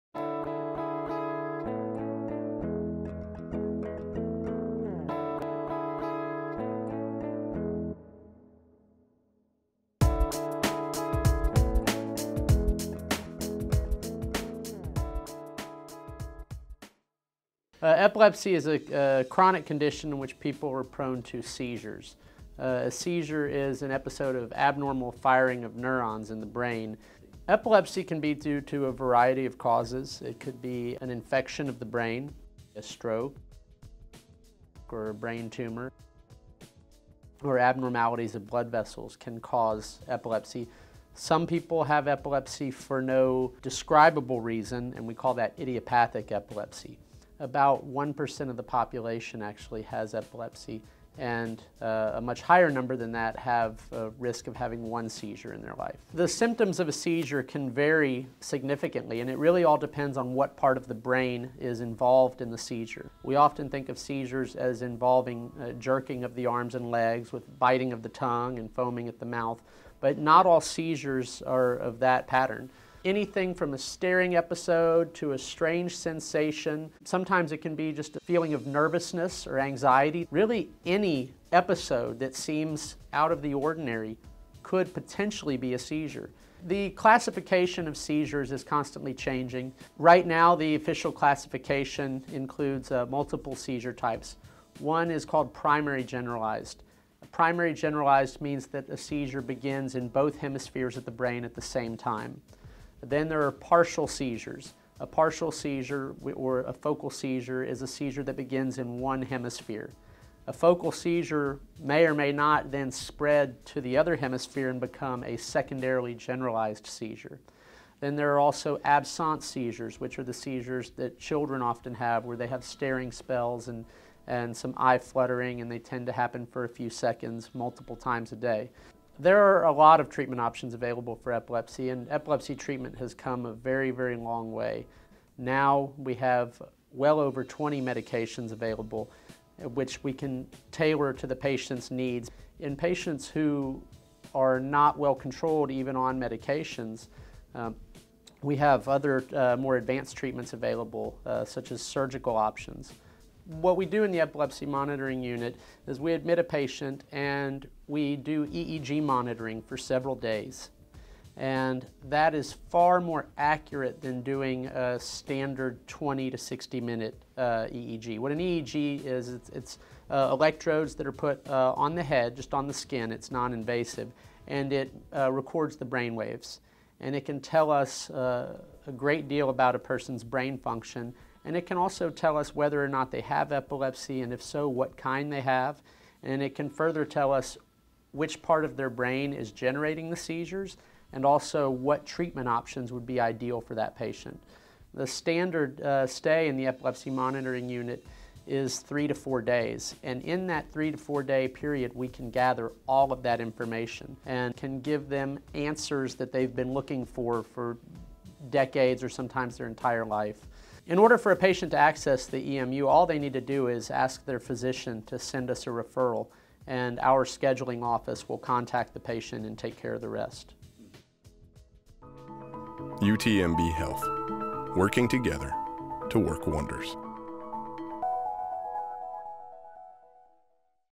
UTMB's Dr. Todd Masel on Epilepsy Symptoms, Diagnoses and Treatments
Epilepsy is a disorder of the brain that causes seizures and can affect people in different ways. Seizures can be mild and barely noticeable, such as staring spells or twitches to the extremities, while others may be violent and harmful to the individual. Since epilepsy and seizures can occur in many different ways to many different people, diagnosing and treating the disorder is often challenging to patients and physicians.

The causes of epilepsy are just as challenging and can be caused by different conditions that affect a person’s brain. Often the cause is unknown, but some conditions that lead to epilepsy are stroke, brain tumor, traumatic brain injuries, head injury, and central nervous system infection. Seizures are the main sign of epilepsy and typically a person is diagnosed with epilepsy when they have had two or more seizures.

A person who has a seizure for the first time should talk to a healthcare provider, such as a doctor or nurse practitioner. The provider will talk to the person about what happened, and look for the cause of the seizure. Many people who have seizures take tests such as brain scans for a closer look at what is going on.